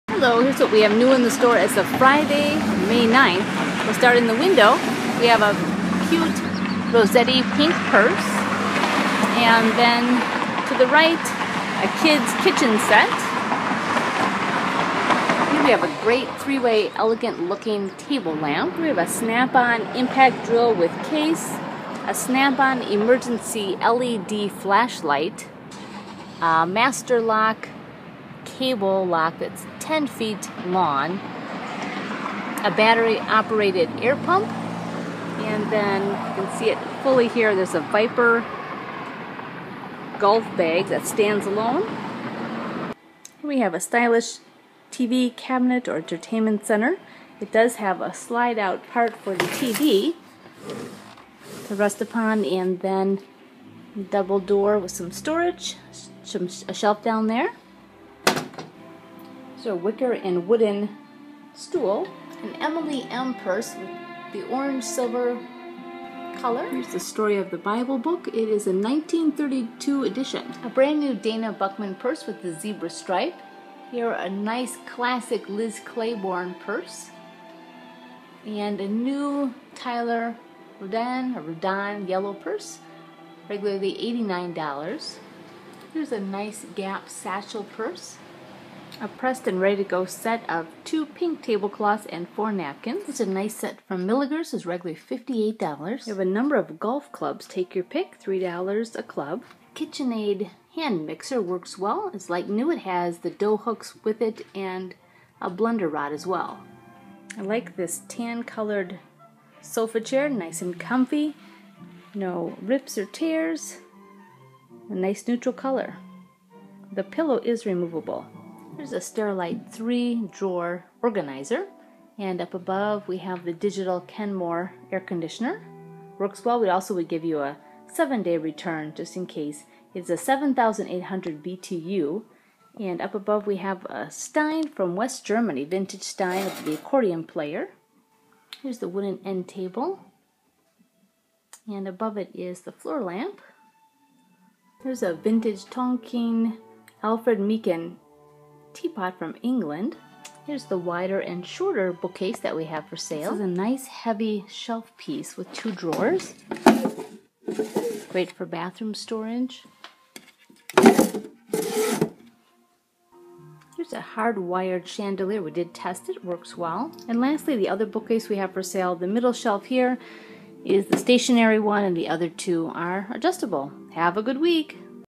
What's New at Cheryl's Family Resale - Milw WI - 5.9.14
Snap-On IMPACT DRILL - Dirty but useful!  Works well! - $150
Kenmore WINDOW AIR CONDITIONER - Digital - 7,800 BTU - Works well! - $99
10 ft. Masterlock CABLE LOCK - $9.50
Snap-On 8-Led Crank EMERGENCY FLASHLIGHT - Reg. $40.99 - $14
Battery AIR PUMP - Great for pumping up your air mattresses while camping - Tested! - $7.50
Viper Hippo GOLF BAG - Stands alone by itself - 49.50
Gap SATCHEL BAG - Black Purse - $9.50
Milaeger's Set of 2 Metal Wall Hangings with Glass Bowls - Reg. $58 - $15
2 PINK TABLECLOTHS and 4 NAPKINS - Pressed and ready to go - $12.50
Heavy Duty SHELF - White/Cream with 2 Drawers - Great for bathroom! - $15
Tan Microfiber SOFA CHAIR - $89
Kids' TOY KITCHEN - $15
Wicker & Wooden STOOL - $20
Wooden BOOKCASES -

    Wider & Shorter Vintage Bookcase with 2 Adjustable Shelves - $30
    Taller & Narrower Contemporary Bookcase with 1 Stationary and 2 Adjustable Shelves - $30

Great 3-Way TABLE LAMP - Stylish / Elegant / Nice! - Works well! - $29
1932 Hurlbut's STORY OF THE BIBLE Book - Selling for $39 + Shipping on Amazon - $15
Gold-Colored Chandelier - Tested: Works well! - $12.50
Floor Lamp - Works well! - 3-Way Bulb - $19
Vintage BEER STEIN from WEST GERMANY - Accordion Player #4156 - $15
KitchenAid HAND MIXER - Works well! - With 2 Dough Hooks & Blender Rod - Like new! - Reg. $34 - $15
Vintage TEAPOT - Excellent TONQUIN Alfred Meakin, Made in England - $15
Beautiful TV Cabinet / Stand / ENTERTAINMENT CENTER - Modern, Will Fit a Pretty Large TV - Excellent Condition! - Has Slide-Out TV Holder, Glass Bottom Doors with Shelves - Stylish! Solid wood! - $159
Sterilite 3 Drawer ORGANIZER - White & Clear - $9.50
Wooden END TABLE - Current piece, but shows some wear, so the price is only $18
Men's / Women's GOLF CLUBS - Variety - Only $3 each!
Large Rosetti Pink PURSE - Cute! - $15
Emilie M. Orange /Silver PURSE - Smaller size, Stylish! - $21
NEW Dana Buchman PURSE with Zebra Stripes Print - Reg. $80+ - $29
NEW Tyler Rodan Yellow PURSE - Reg. $89 - Very pretty and summery! - $33.50
Nice! Classic LIZ CLAIBORNE Red PURSE - $34